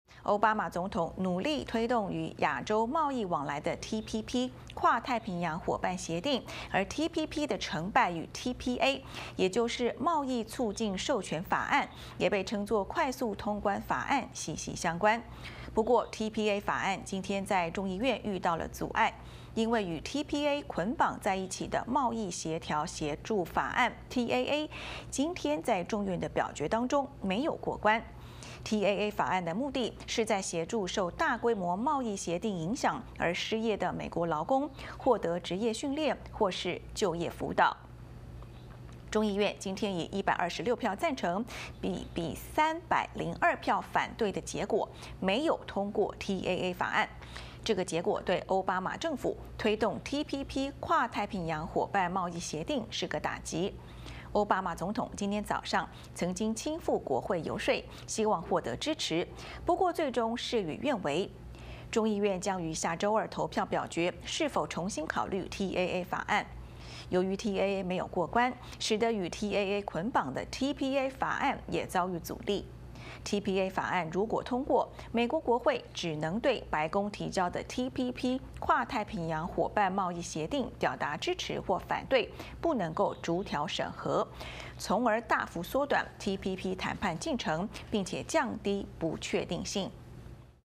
[貿易相關法案失利] 洛城18台晚間新聞 06122015
歐巴馬總統努力推動與亞洲貿易往來的TPP跨太平洋夥伴協定，與TPA，也就是貿易促進授權法案，也被稱作快速通關法案的成敗息息相關。不過TPA法案今天在眾議院遇到阻礙，因為與TPA捆綁在一起的一項法案TAA今天在眾院表決中沒有過關。TAA法案的目的是在協助受大規模貿易協定影響而失業的美國勞工獲得職業訓練或是就業輔導。

洛城18台晚間新聞記者 姜爾蓓 報導